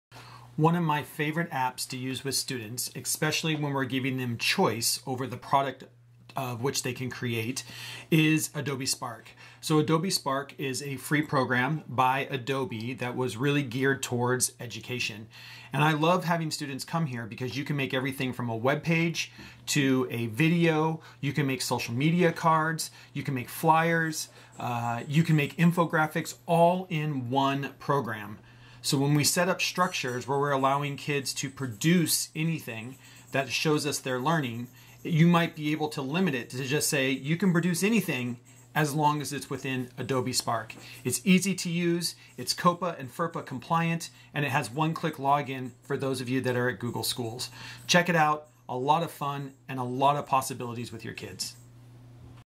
Adobe Spark Intro
A great website for educators to use with students in giving them choice over a product they can create in one place. Everything from video, webpages, social media cards, and infographics. Adobe Spark does it all.